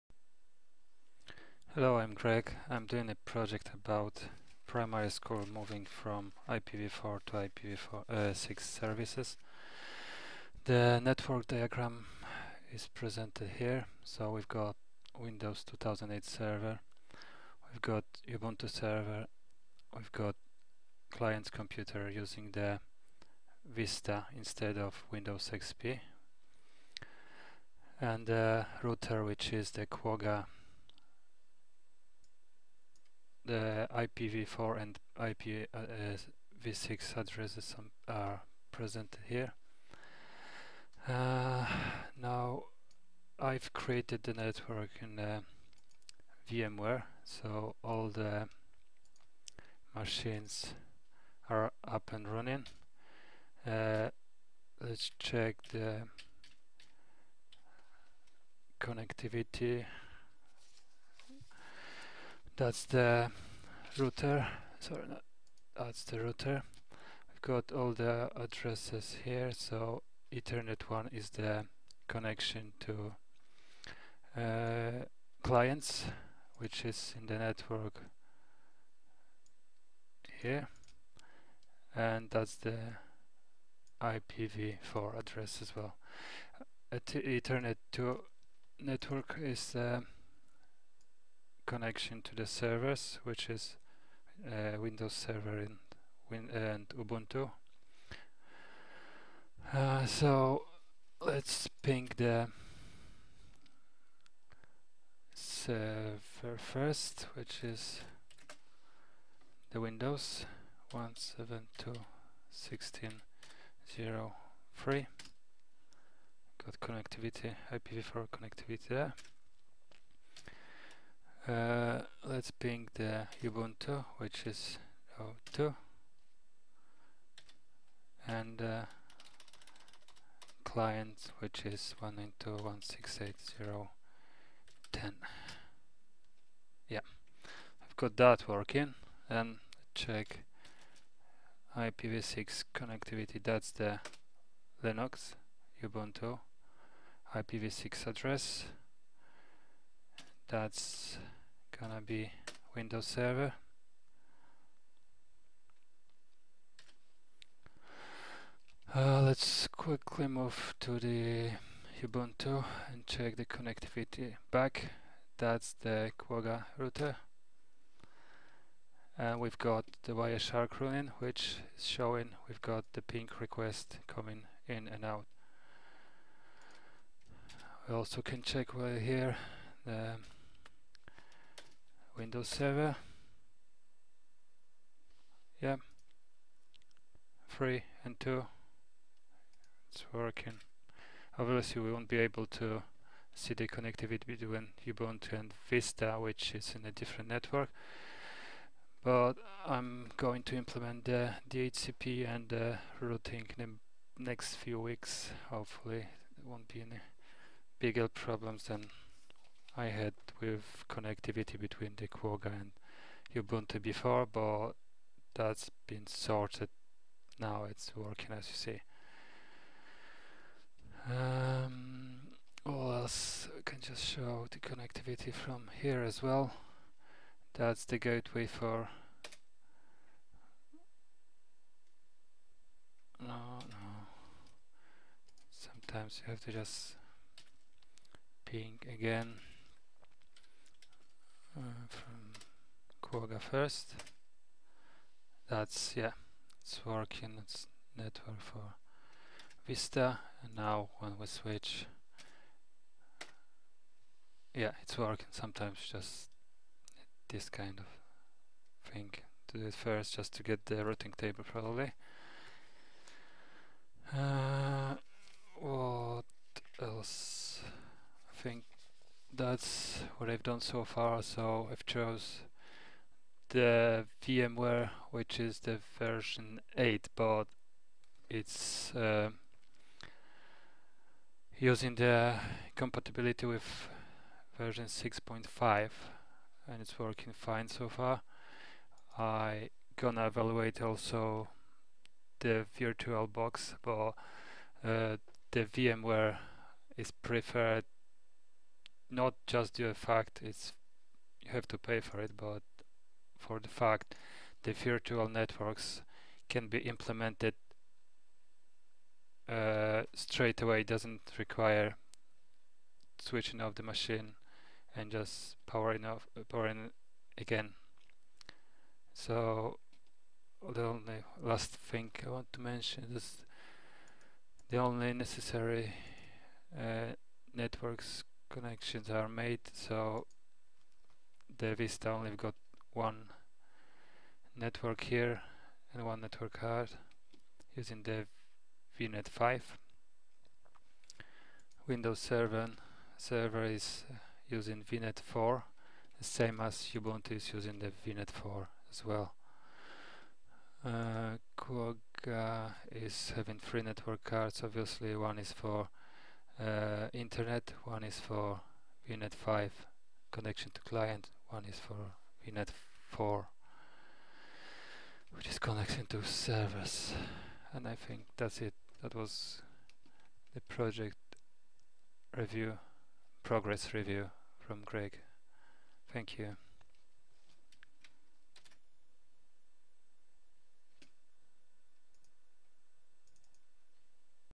IPv6: DNSv6, DHCPv6, Final Project - Primary School scenario
The Project is about Primary School moving their services from Ipv4 to IPv6. Video Shows network topology and running VM machines with both IPv4 and IPv6 connectivity. Next step within the project wil